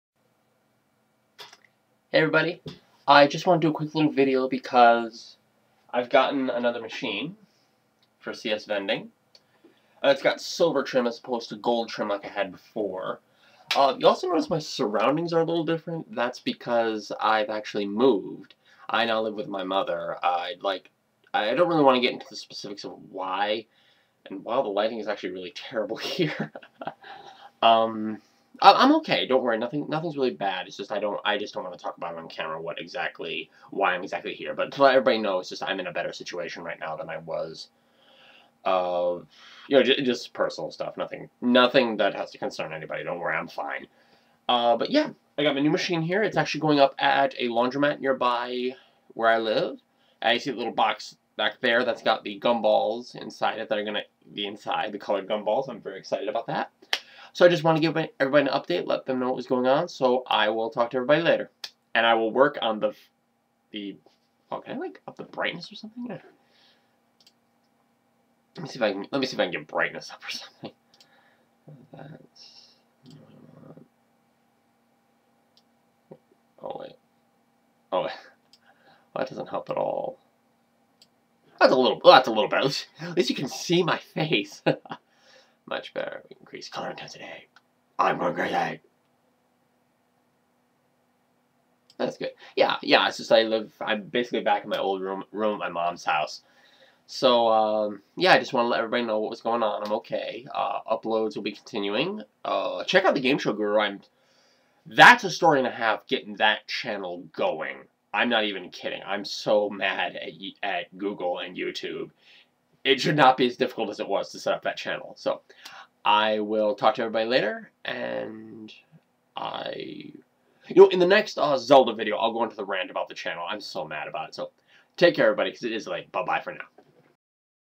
CS Vending, 3/18/2012
I just wanted to let people know about machine #2 in my business. Also, just s few things on what's been happening!